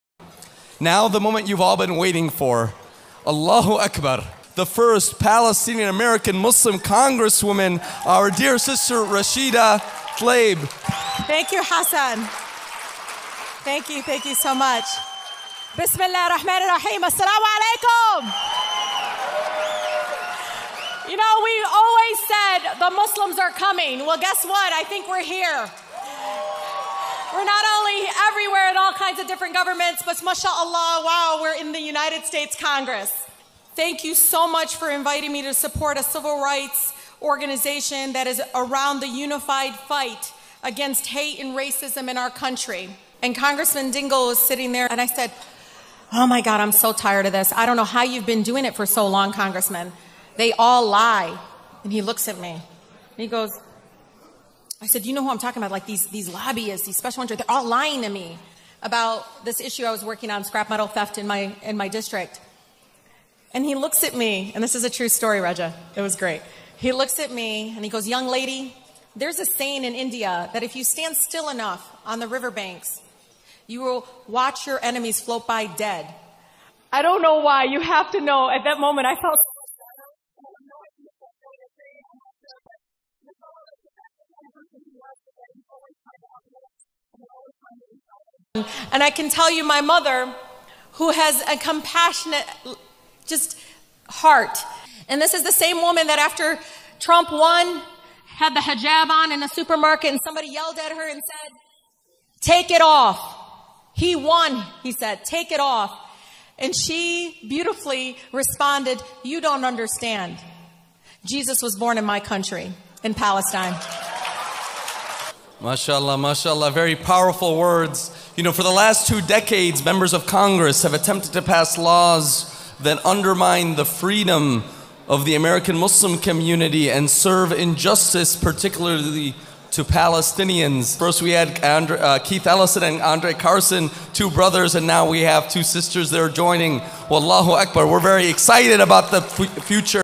Rashida Tlaib presents at HAMAS-linked CAIR-Chicago Conference
Rashida explains that envisioning her enemies (Lobbyists & Special Interests groups) DEAD brings her joy & inspiration

CAIR is established by U.S. Muslim Brotherhood’s “Palestine Committee” which is Hamas in America